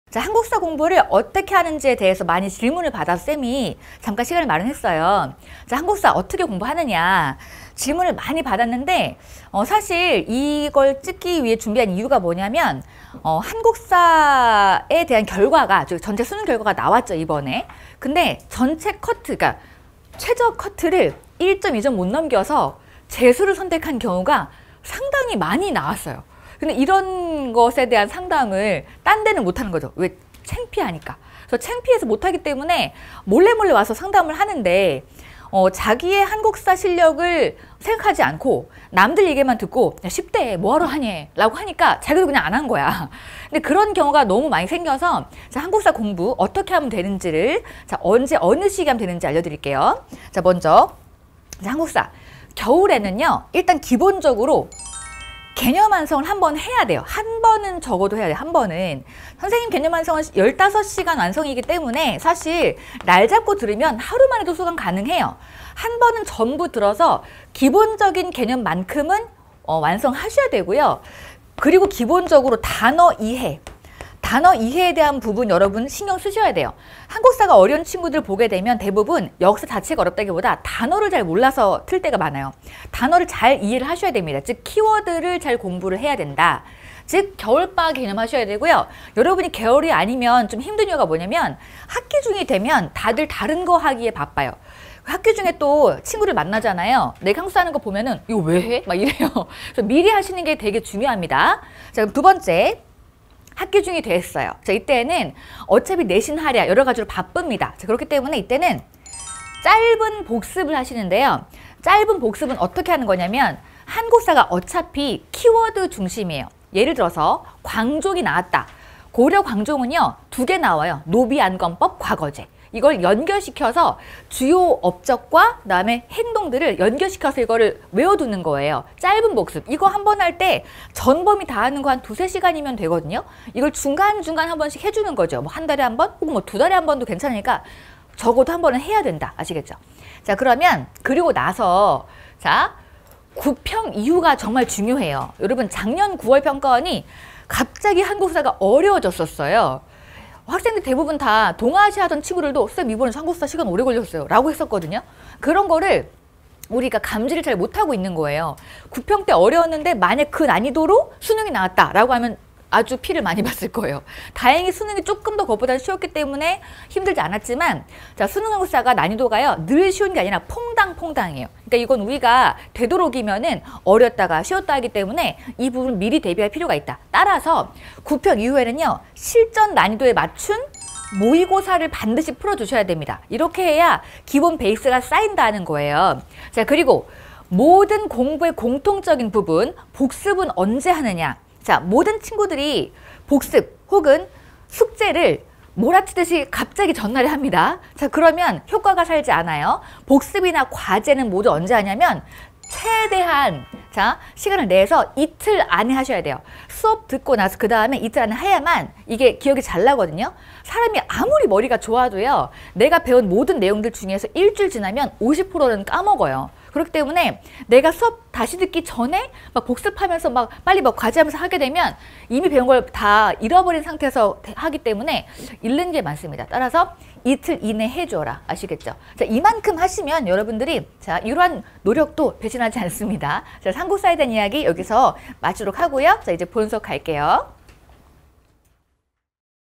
[메가스터디] 한국사 고아름쌤 - 한국사 공부, 어떻게 해야 좋을까?
한국사 공부는 어떻게 해야 좋을까요?
고아름 선생님이 알려드립니다!

1. 겨울에 개념완성 마무리 하기
2. 학기 중에는 짧은 복습!
3. 9평 이후에는 모의고사 풀이!

더 자세한 내용은 영상을 통해 확인해 주세요!

- 한국사 고아름 선생님에 대해 더 자세히 알고 싶다면?